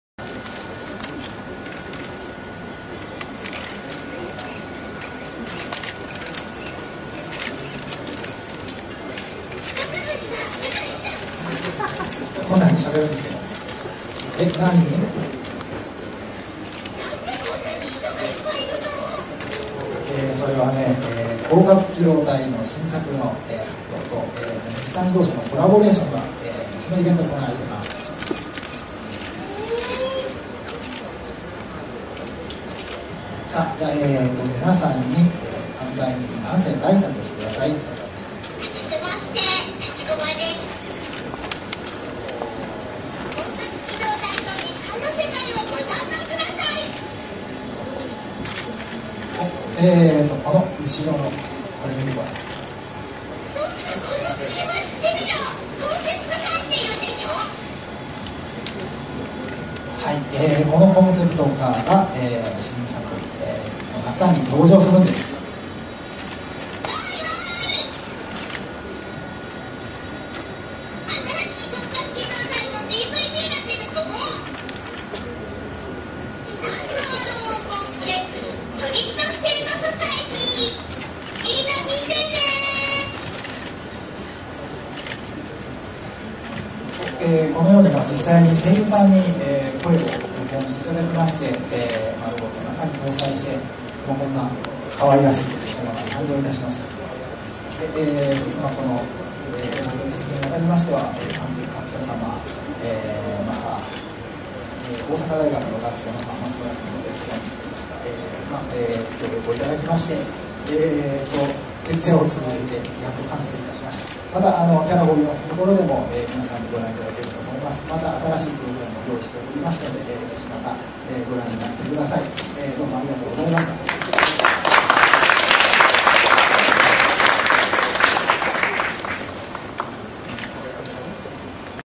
Nissan Tachikoma
Nissan partnered in providing two cars which appeared in Solid State Society in 2006. As part of the promotion, the talking Tachikoma (also seen in a different clip on this channel) was presented alongside a real Nissan car and it happily chatted away in Japanese. Possibly this was the birth of the advertising slogan "The Tachikoma in front of you is a Nissan" but don't quote us on that, because we don't speak Japanese.